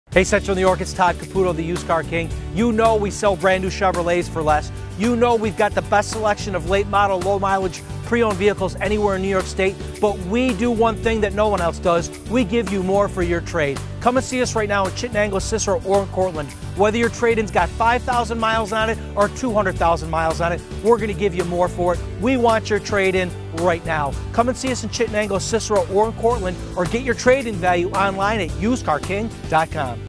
We Want YOUR TRADE during our Summer Sales Drive Event!
It doesn't matter if your vehicle has 5,000 miles on it or 200,000 miles! We want your trade! Bring your vehicle in to any Sun Auto Location in Chittenango, Cicero or Cortland and we'll appraise it on the spot. You don't even have to purchase another vehicle from us! But if you're looking for another vehicle, we have over 1,000 pre-owned & New Chevys in stock to choose from, and like always, Sun gives you MORE FOR YOUR TRADE!

Plus, EVERY vehicle purchase comes with our VIP Perks Package good for the life of your vehicle…..Free Car Washes, Free yearly NY State Inspections & a Free Birthday Detail or Oil Change!